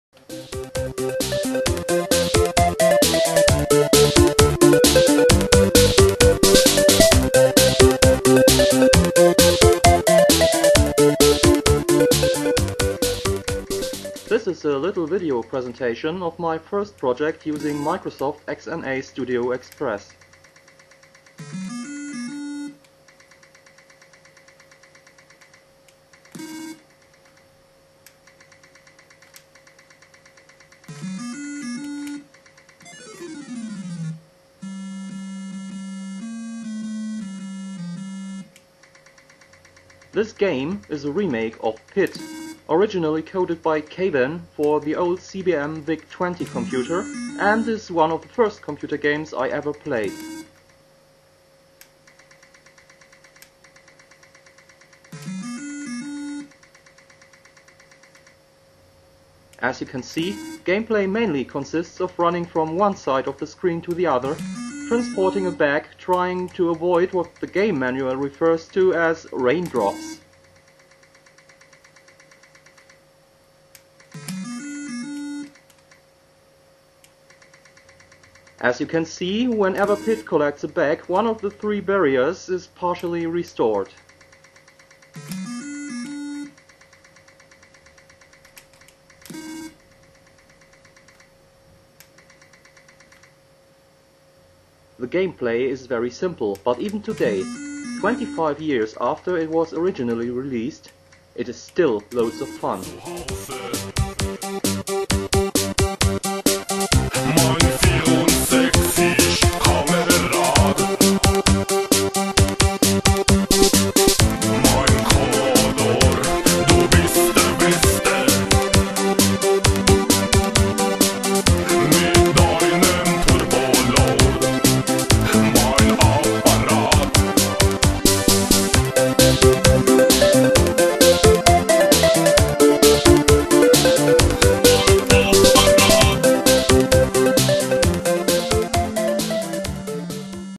Pit XNA remake
A little presentation video of my first xna game, a remake of the 25 years old game 'Pit', originally developed for the CBM VIC-20 by Kavan.
The music in this video is by Tomas Danko.